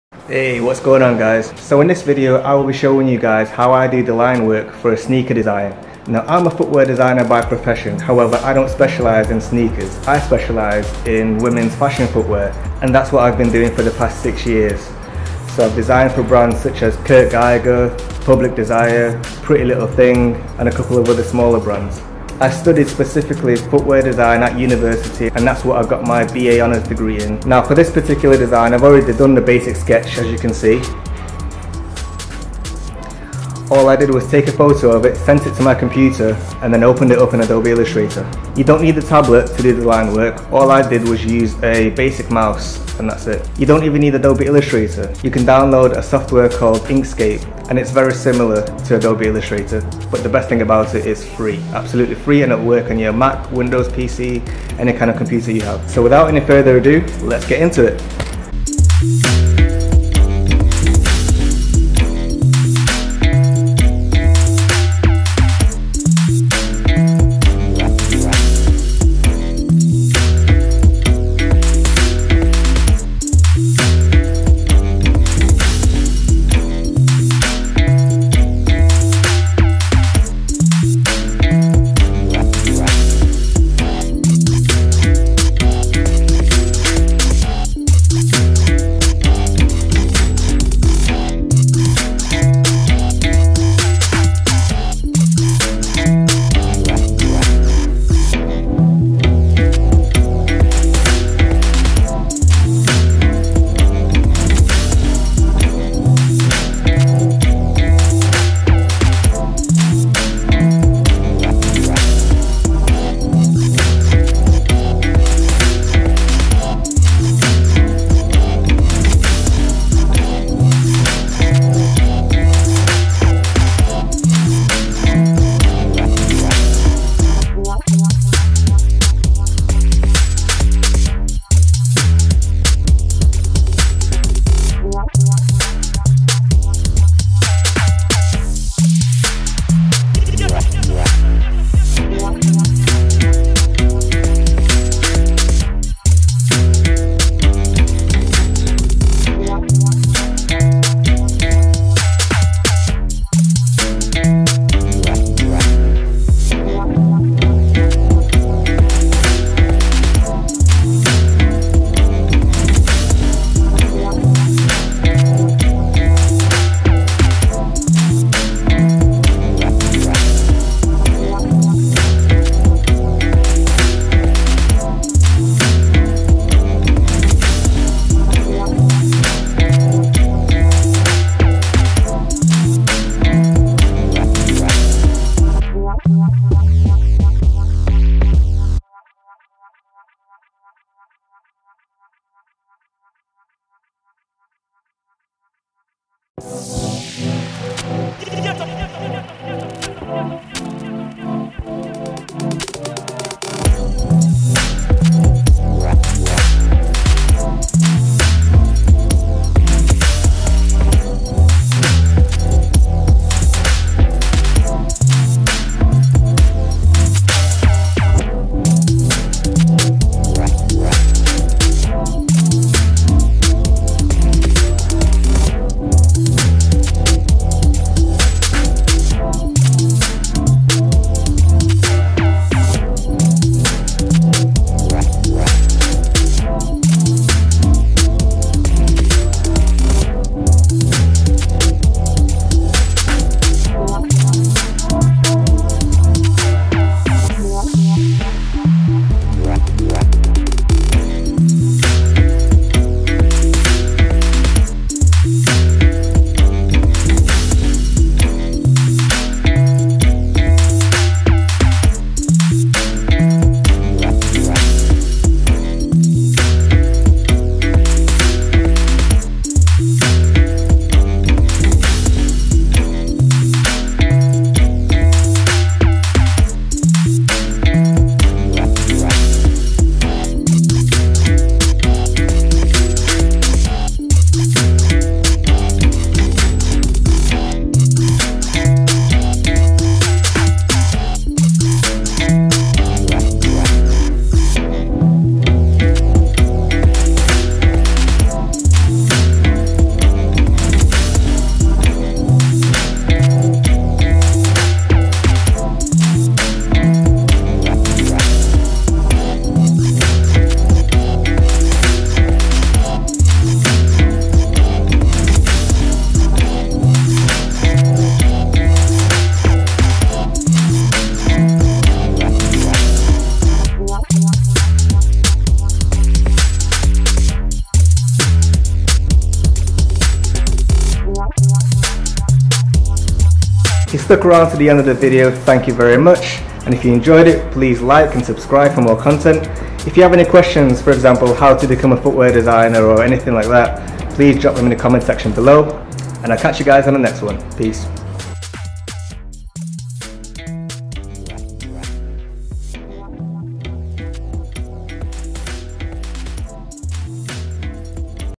SNEAKER DESIGN by a Footwear Designer Using Adobe Illustrator and a Mouse - TIME LAPSE
So in this video I will be doing the line work for a sneaker which I sketched earlier. I did this using Adobe Illustrator and a mouse so you don't need a tablet.

If you don't  have Adobe Illustrator you can download a vector creating software similar to Illustrator called Inkscape for free!